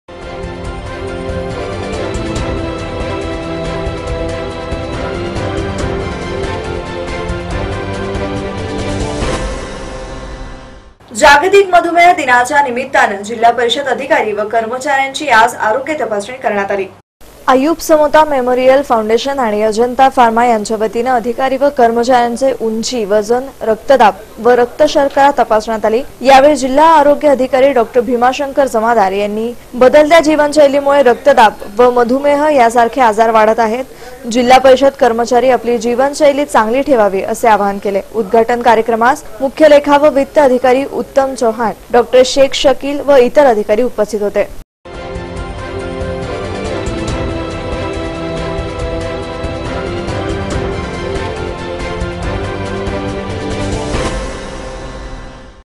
Aurangabad जागतिक मधूमेह दिनाच्या निमित्ताने जिल्हा परिषद अधिकारी व कर्मचारींची आज आरोग्य तपासणी
Aurangabad :-  जागतिक मधूमेह दिनाच्या निमित्ताने जिल्हा परिषद अधिकारी व कर्मचारींची आज आरोग्य तपासणी करण्यात आली.